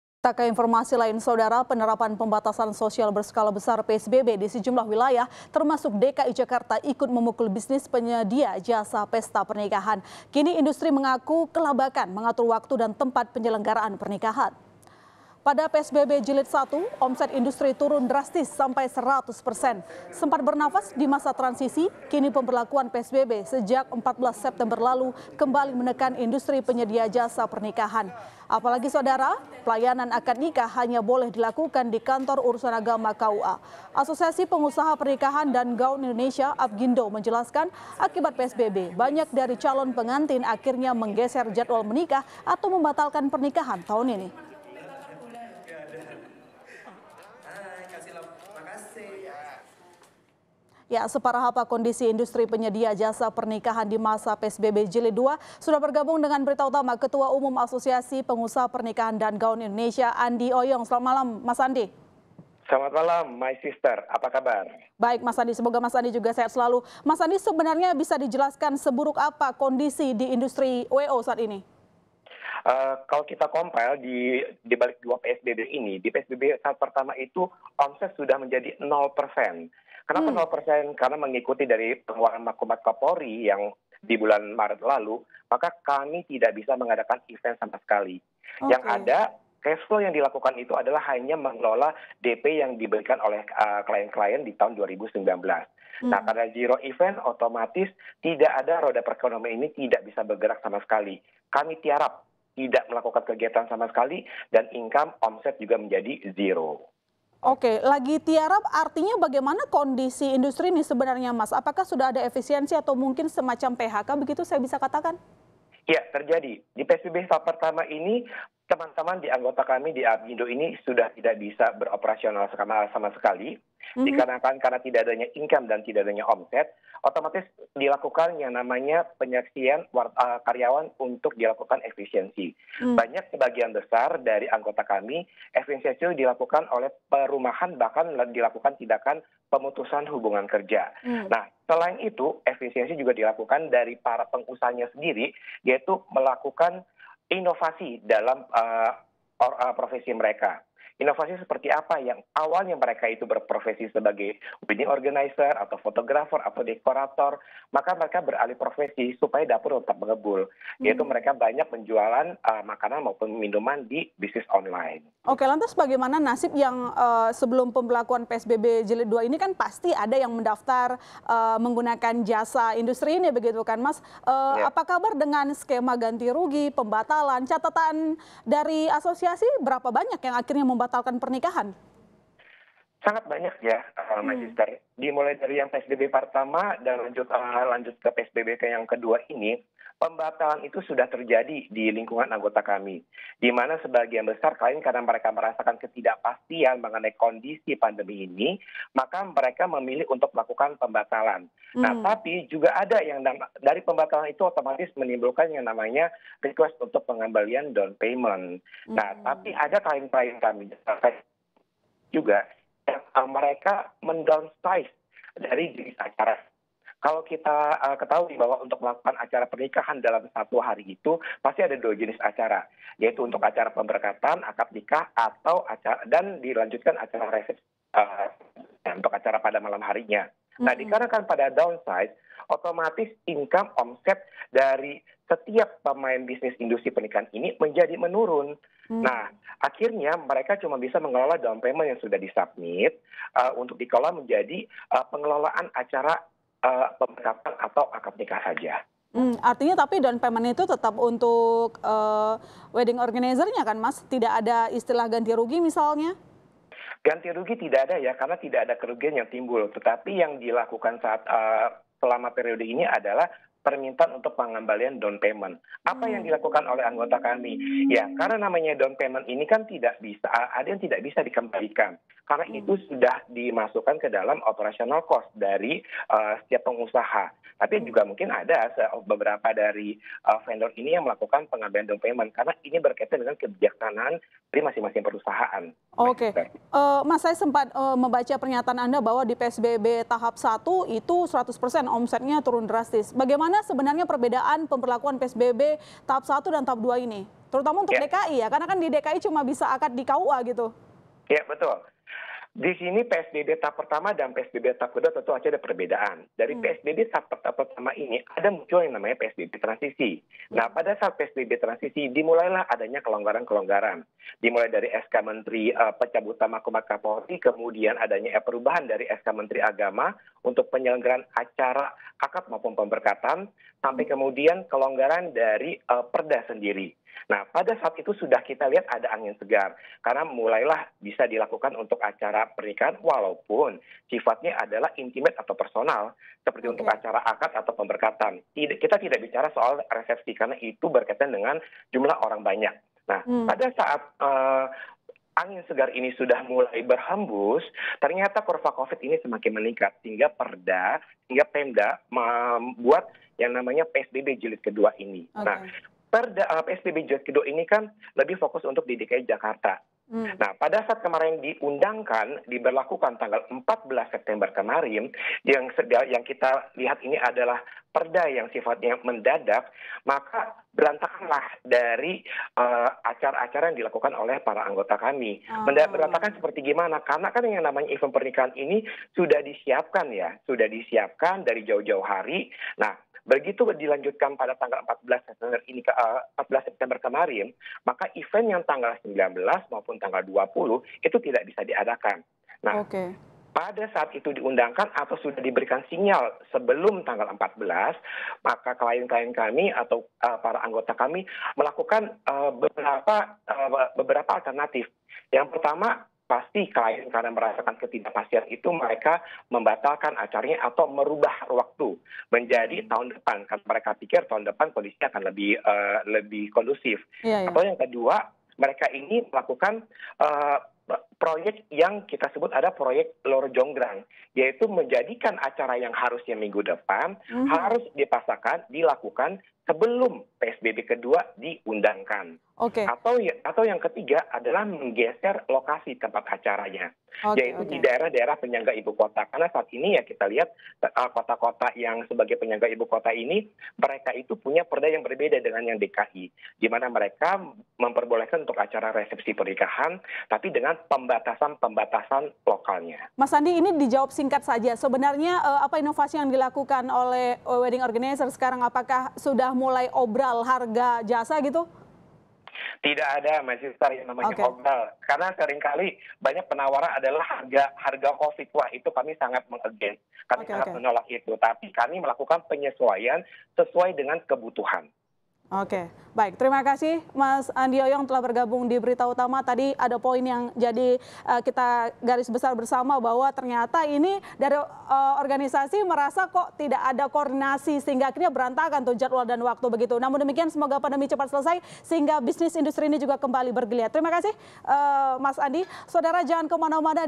PSBB JIlid 2 DKI Jakarta, Bisnis Wedding Organizer Kian Lesu
JAKARTA, KOMPAS.TV - Penerapan pembatasan sosial berskala besar , psbb di sejumlah wilayah termasuk DKI Jakarta , ikut memukul bisnis penyedia jasa pesta pernikahan.

Kini industri mengaku kelabakan mengatur waktu dan tempat penyelenggaran pernikahan.

Pada psbb jilid satu, omset industri  turun drastis sampai 100 persen.

Sempat bernafas di masa transisi, kini pemberlakuan psbb sejak 14 September lalu , kembali menekan industri penyedia jasa pernikahan.

Apalagi,  pelayanan akad nikah hanya boleh dilakukan di kantor urusan agama, KUA.

Asosiasi Pengusaha Pernikahan dan Gaun Indonesia , APPGINDO menjelaskan, akibat psbb, banyak dari calon pengantin akhirnya menggeser jadwal menikah atau membatalkan pernikahan tahun ini.

Tidak hanya bisnis "wedding organizer" yang kian sepi dan lesu, beberapa jenis bisnis lain juga lesu di tengah pandemi corona di Indonesia ini.

Tentunya kita berharap agar pandemi Corona segera berakhir dan para pelaku bisnis dapat kembali membangun rencana bisnis yang menguntungkan.